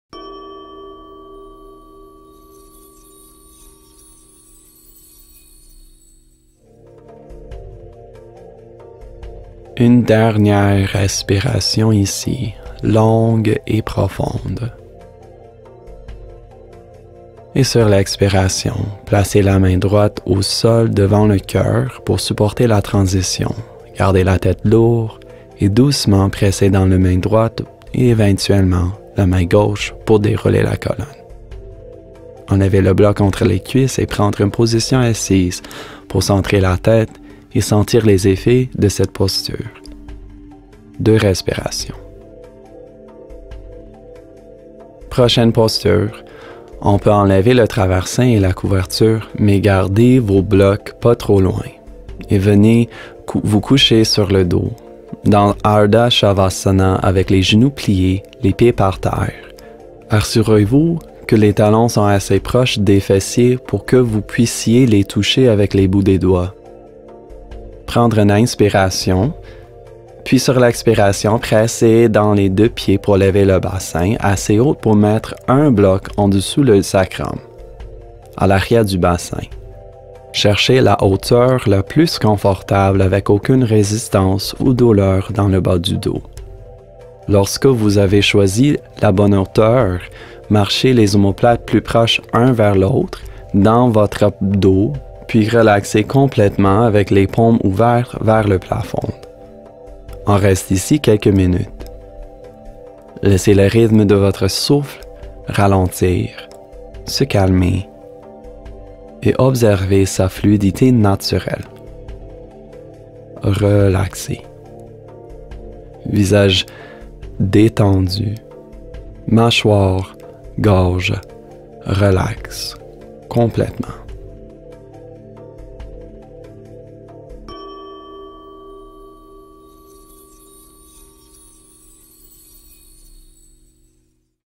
Pratique restauratrice de la Terre 20 avec Andrew Bathory - Mon Yoga Virtuel
(Accessoires: 2 blocs, une couverture, un traversin)- Une pratique restauratrice conçue pour faire sortir la tension dans le corps et l'esprit tout en vous connectant à l’énergie de la Terre.